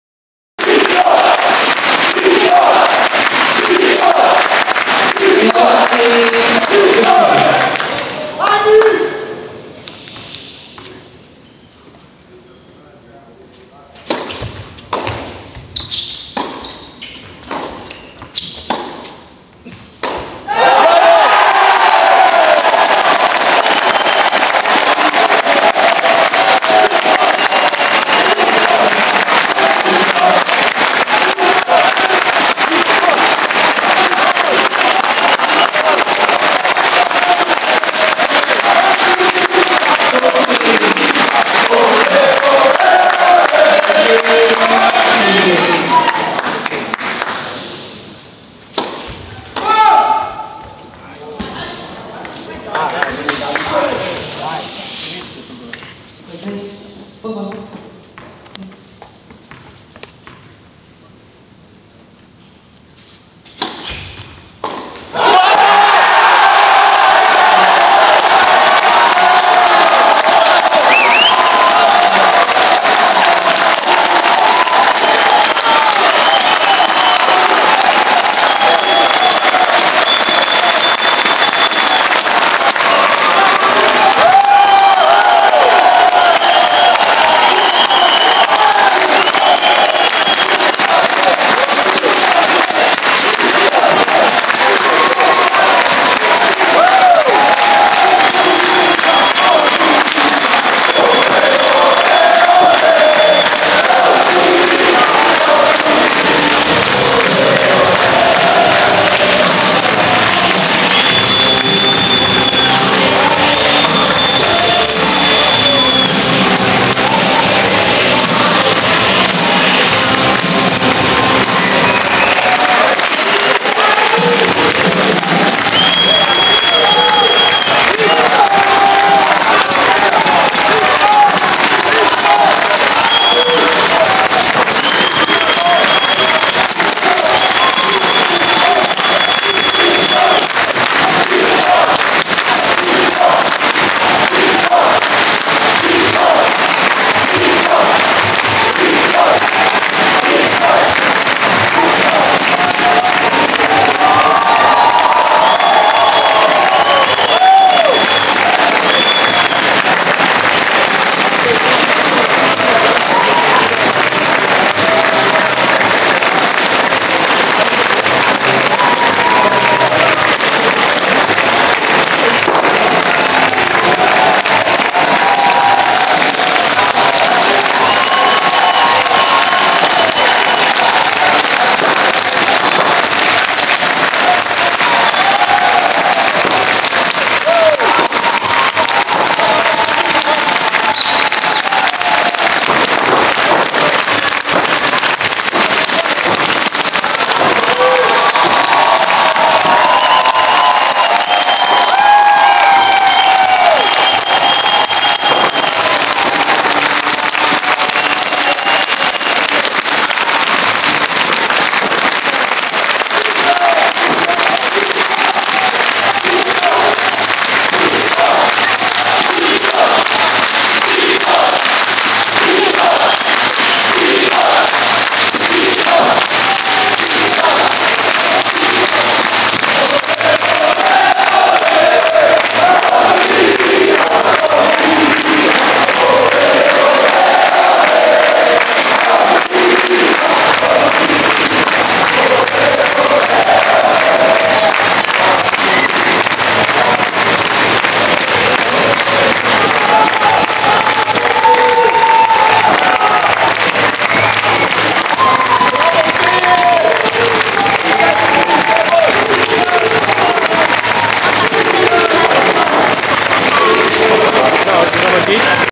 Punctul final al victoriei lui Victor Crivoi in fata lui Harri Heliovaara
Punctul decisv plus atmosfera de dupa meciul dintre Victori Crivoi si Harri Heliovaara, meci decisiv care s-a incheiat cu victoria romanului, scor 4-6, 7-6 (5), 6-7 (2), 6-3, 7-5.